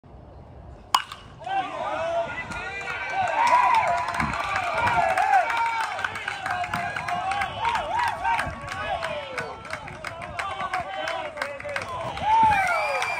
Dylan Oblepias (2025) Fulshear (TX)-Offense (Left Center Gap Double 2 RBI)
Dylan Oblepias
2025
Fulshear (TX)
Offense - Left Center Gap Double 2 RBI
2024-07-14